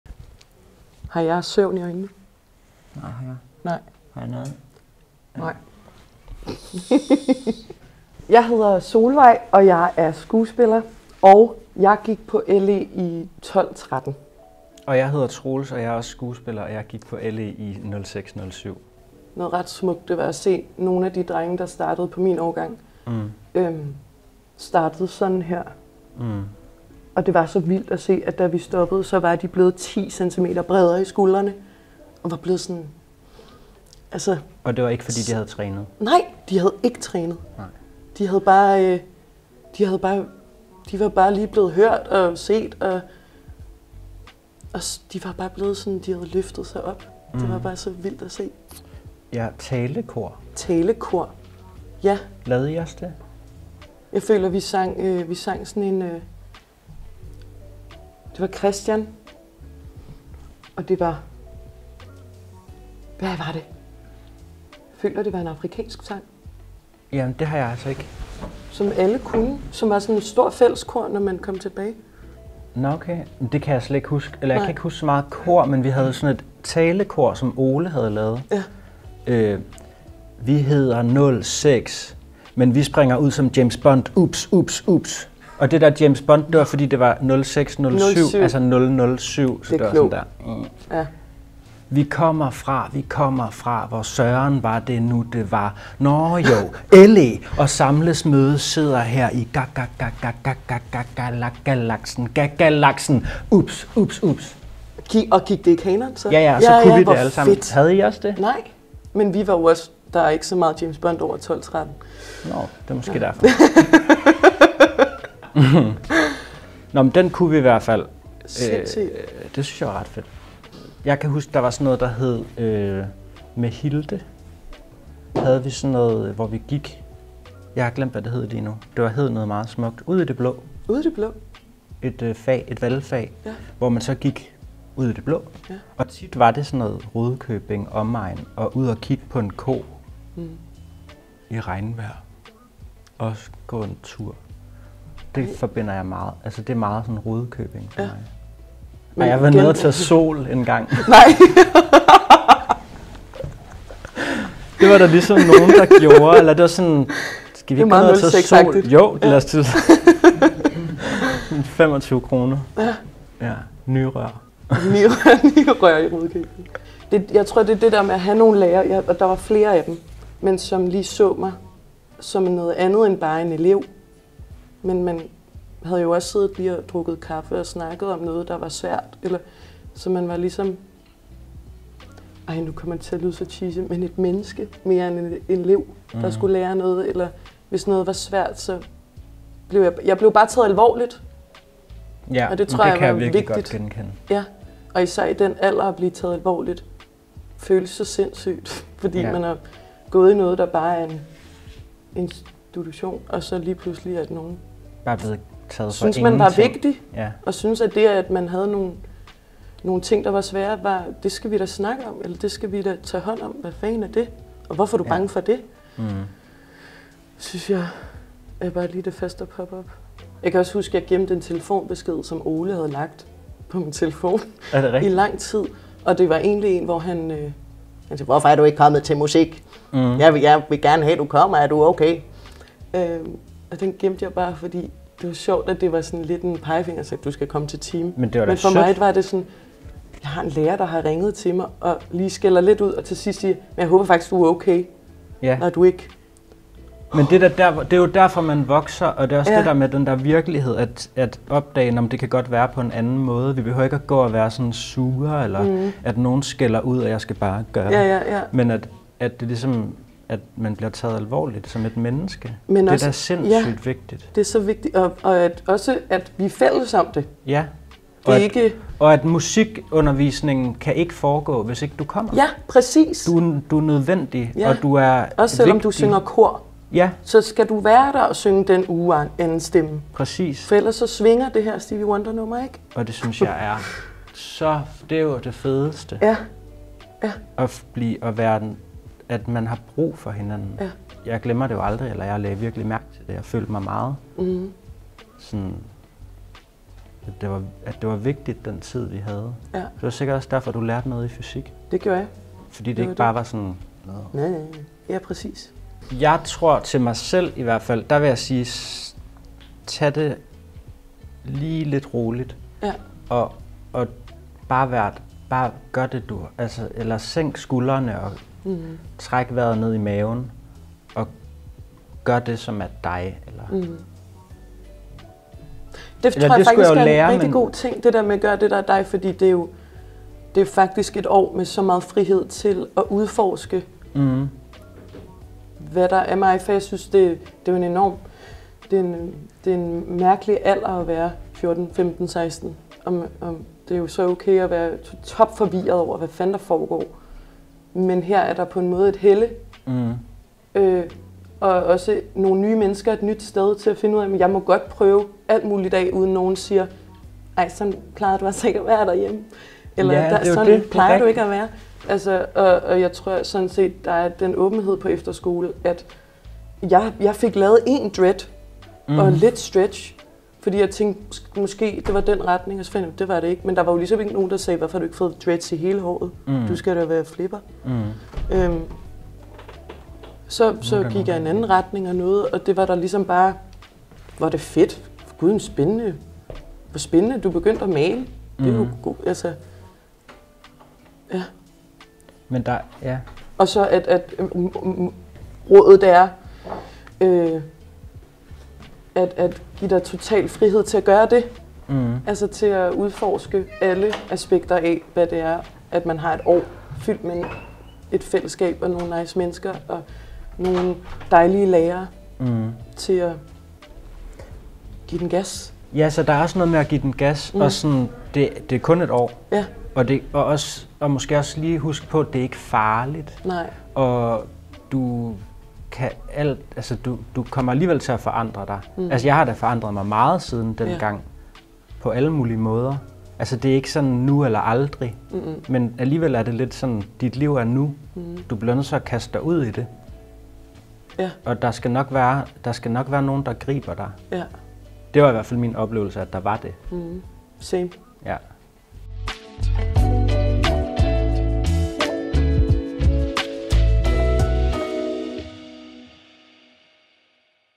Langelands Efterskoles 50 års jubilæum, film 4
Ga-ga-ga-ga-ga-ga-ga-ga-ga-galaksen - oops, oops, oops
Skuespillerne Solvej Sonne og Troels Kortegaard er begge tidligere elever på Langelands Efterskole. I samtalesofaen mindes de to Galaksen med dens fællessang og talekor, hvilket får Troels til spontant at bryde ud i en talekorsgalaksesang, som har ligget gemt i hans hjerne siden 2007. Hør Troels’ fabelagtige fremførelse, og lyt til hans og Solvejs samtale om blandt andet oplevelsen af for første gang at føle sig taget alvorligt af voksne, at være vigtig og nødvendig for andre og at kigge på en ko og tage sol i Rudkøbing.